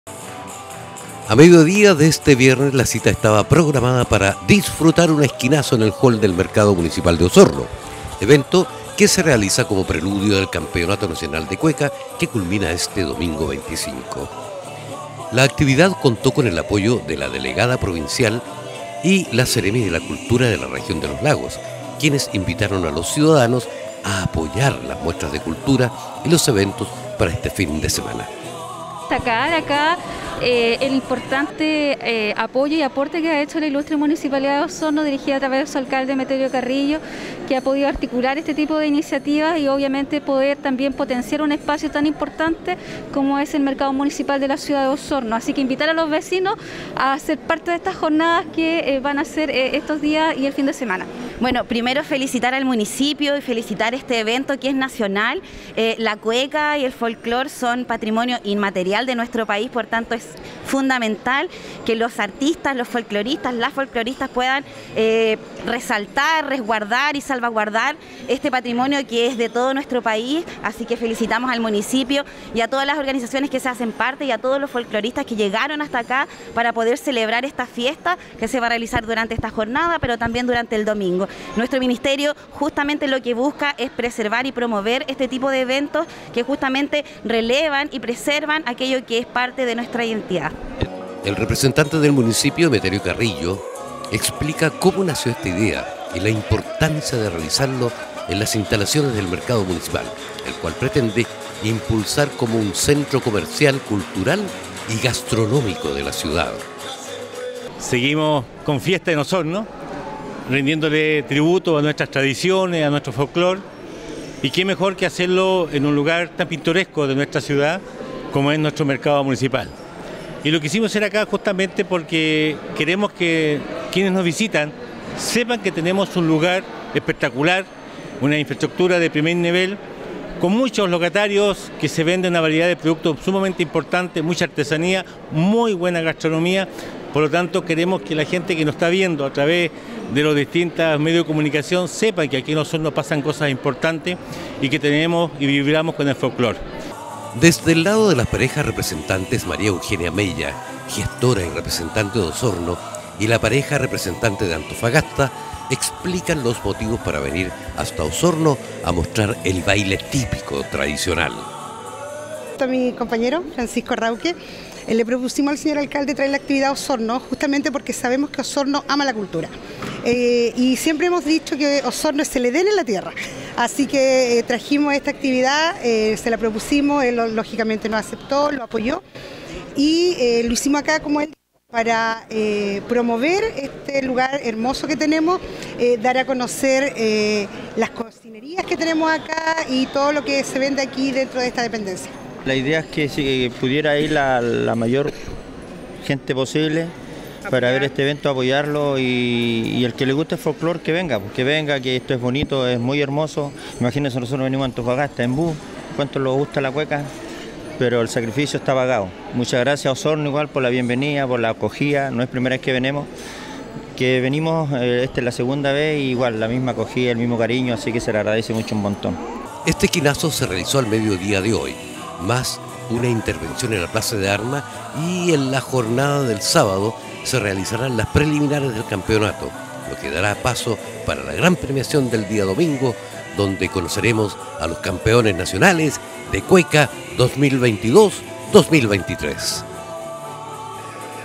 Esquinazo da inicio a Campeonato Nacional de Cueca en Osorno
Al medio día de este viernes la cita estaba programada para disfrutar de un esquinazo en el hall del mercado municipal de osorno, evento que se realiza como preludio del Campeonato Nacional de Cueca que culmina este domingo 25.